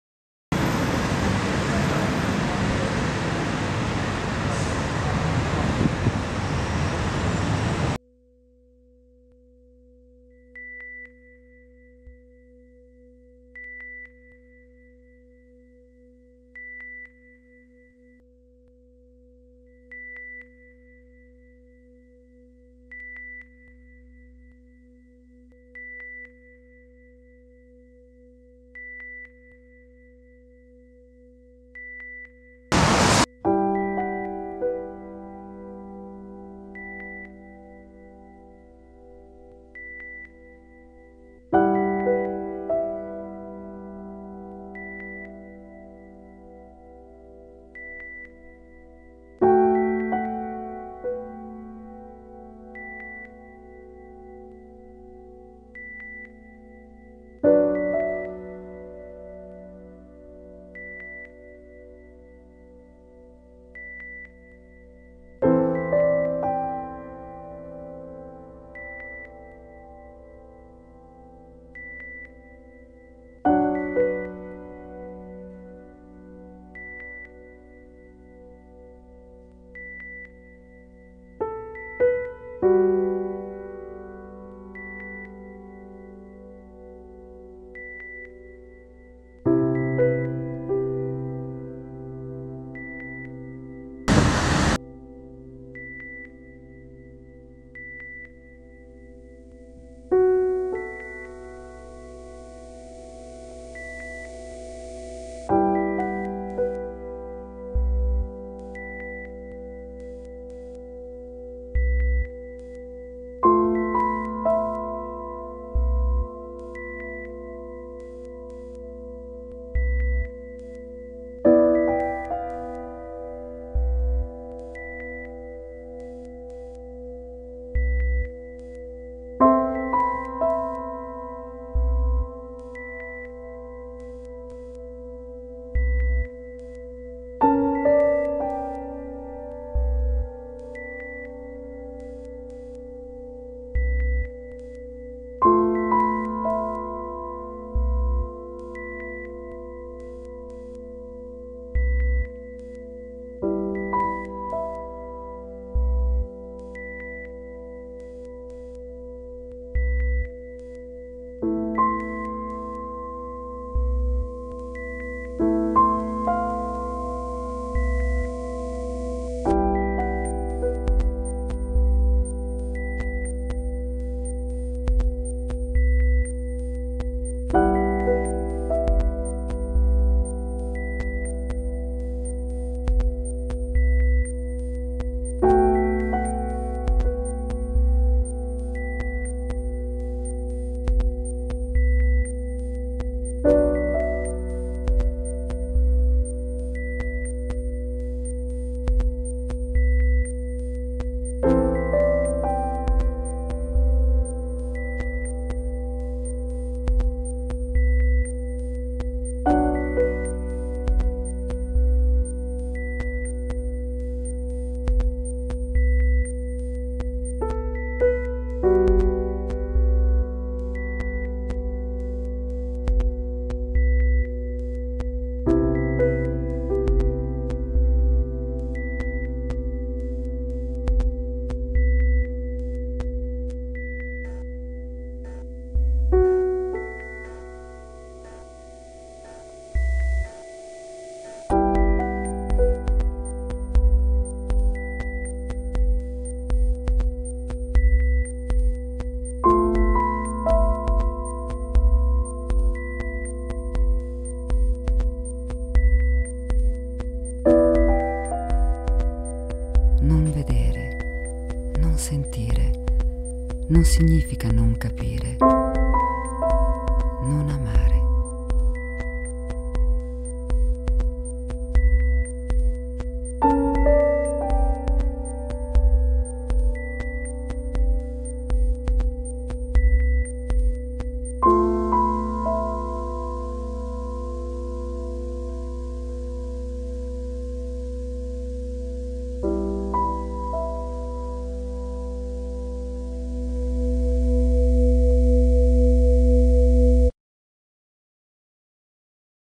CHIUDI I MIEI OCCHI
Un video di Juan Diego Puerta Lopez e Andrés Arce Maldonado per la Lega del Filo D'Oro
con:
Rosalinda Celentano e Simona Borioni

ROMA - ITALY
2012